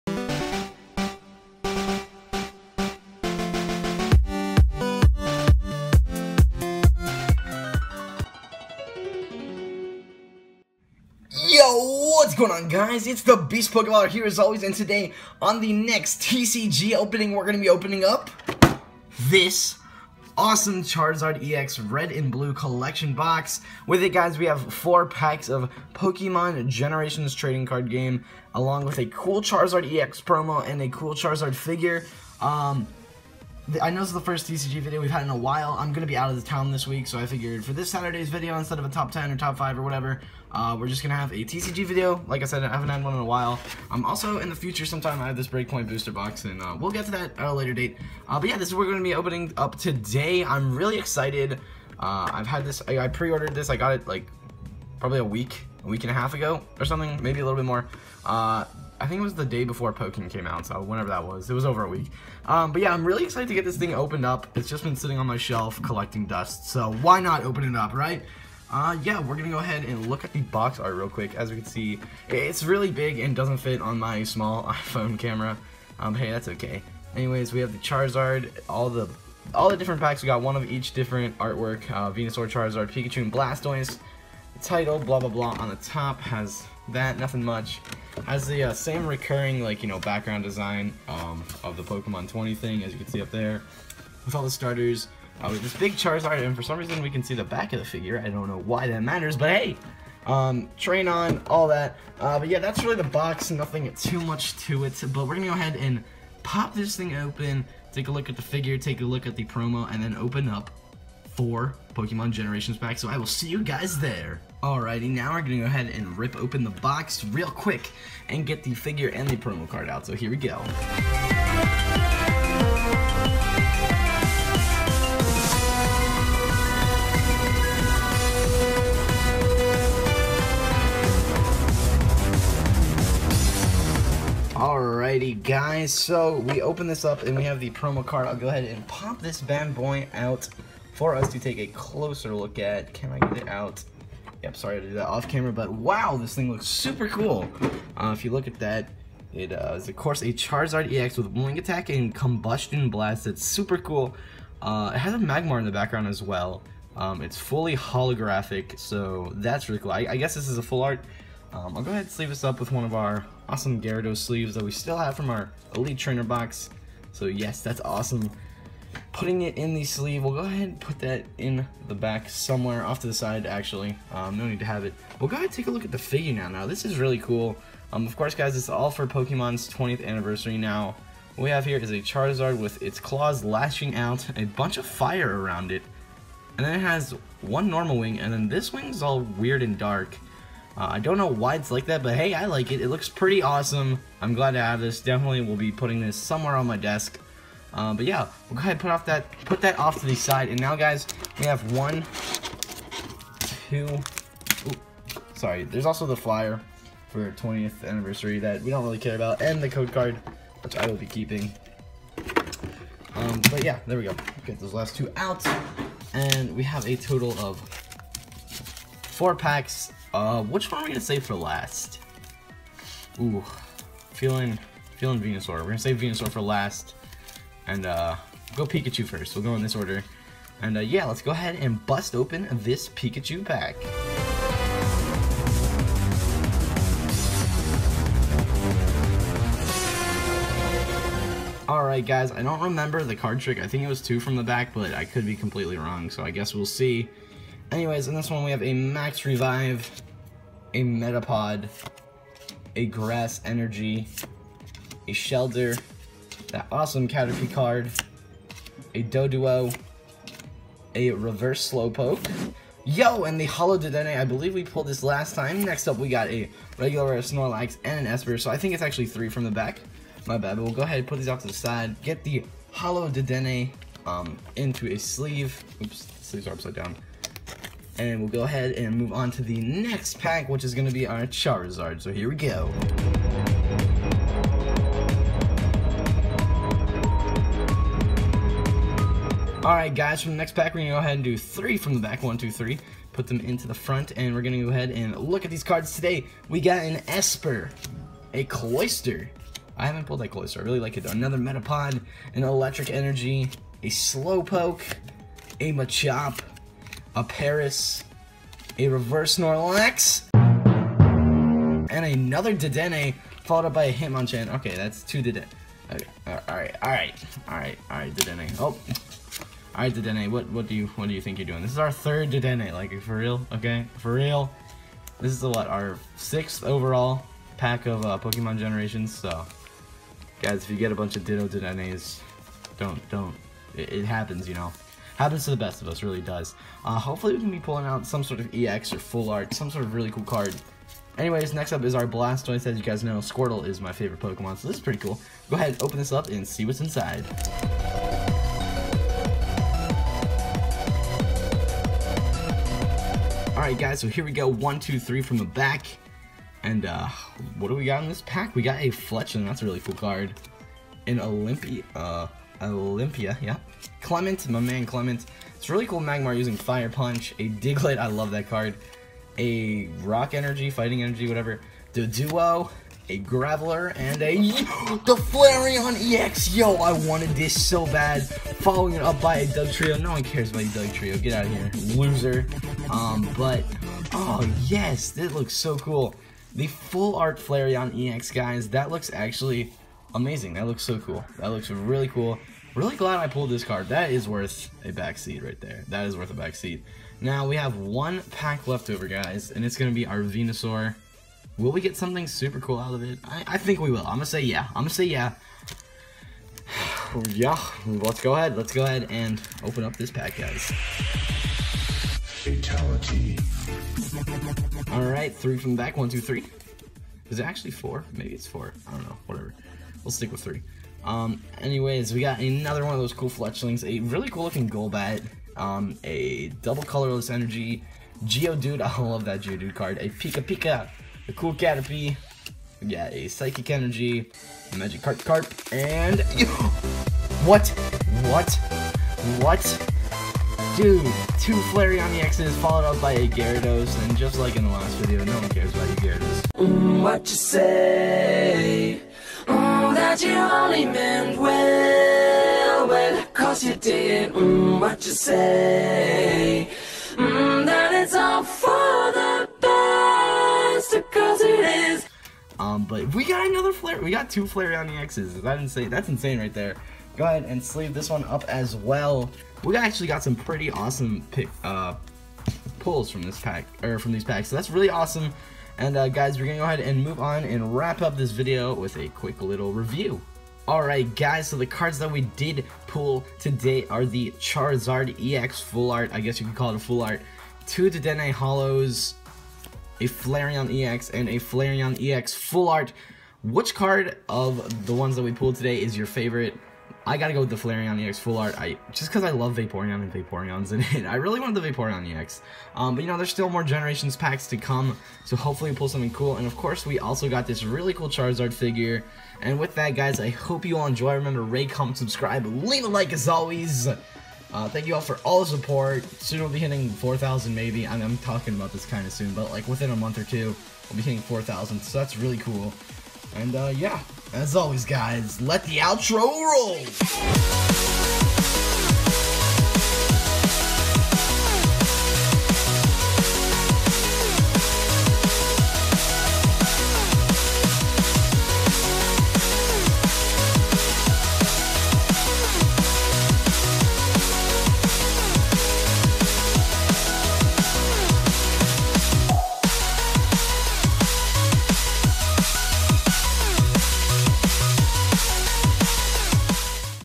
[INSANE PULLS!] Opening a Charizard EX Red and Blue Collection Box! 4 Packs of TCG Generations!
Sorry guys, no top 10/5 this week :P I'm out of town, so I didn't have time to make one, but there should be a top 5 next Saturday! Have a nice day guys :D